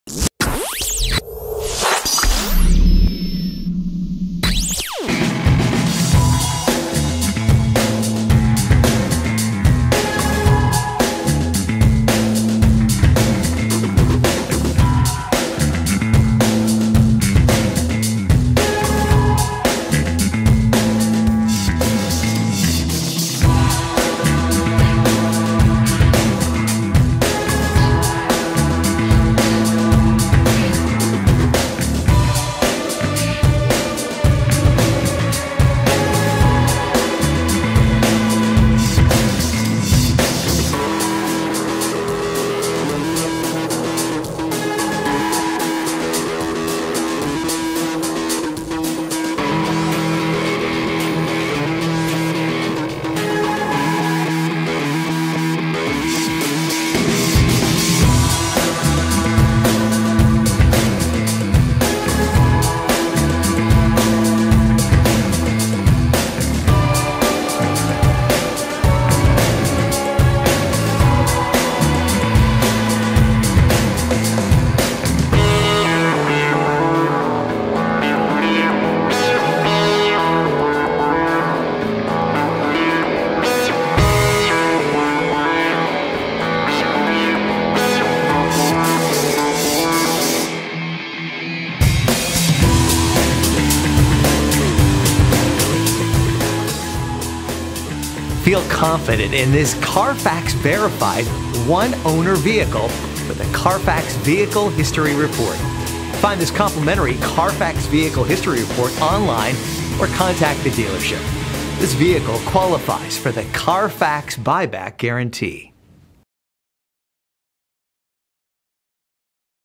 Used 2018 Toyota Camry LE, Conshohocken, PA HY7330
Conicelli Hyundai Genesis

CarFax 1-Owner, LOW MILES, Value Priced below the market average! -Backup Camera -Bluetooth -Aux. Audio Input ABS Brakes -Power Seat -Automatic Headlights -AM/FM Radio and many other amenities that are sure to please. Based on the excellent condition of this vehicle, along with the options and color, this Toyota Camry is sure to sell fast. -Front Wheel Drive -CARFAX 1-Owner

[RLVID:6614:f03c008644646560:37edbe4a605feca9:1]